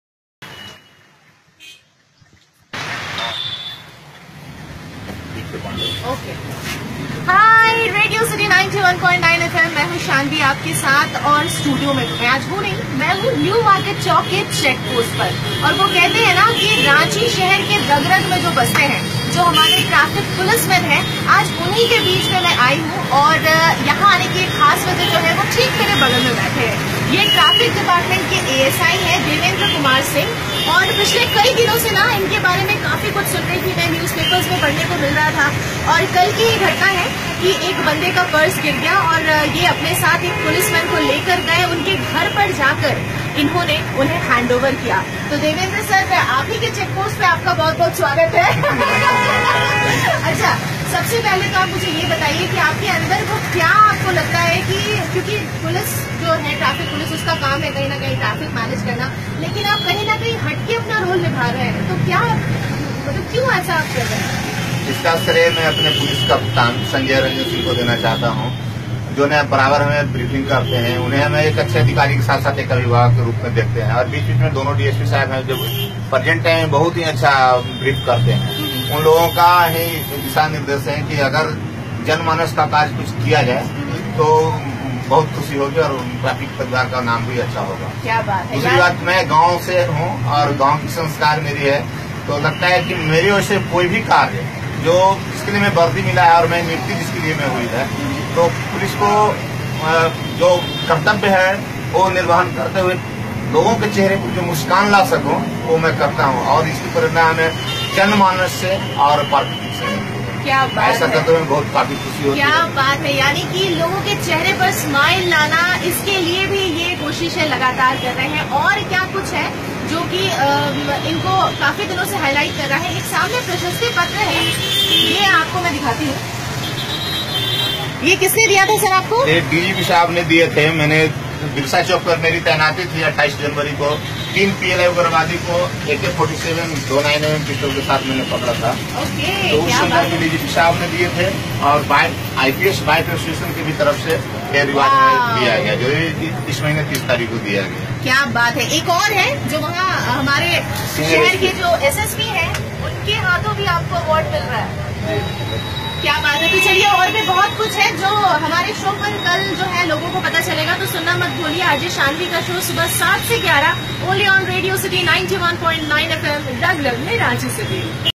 ASI Police. Davendra Kumar Singh from Ranchi JH Making people Smile
He has done a most wonderful job of humanity since he has reported in Birsa Munda Chowk, RANCHI, JH. Please listen to his own word.
Please watch and share it to all. make it viral